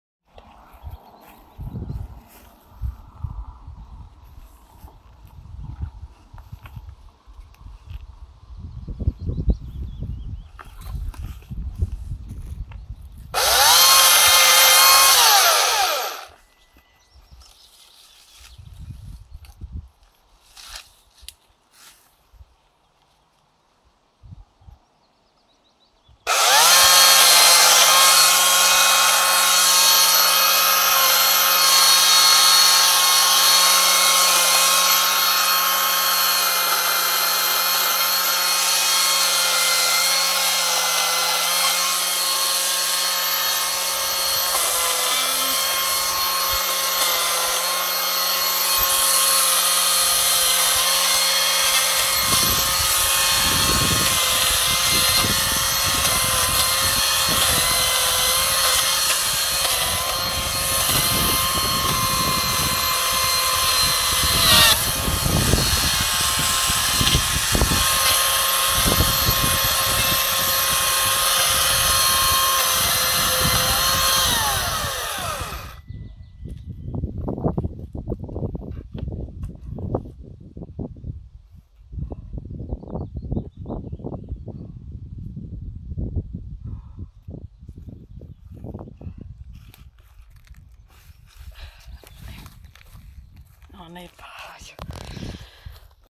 Maassa olevan kuusen oksien harvennusta.
Kaadettua puuta on paitsi helppo harventaa niin siitä voi saada myös hyvän istumapaikan. 🌲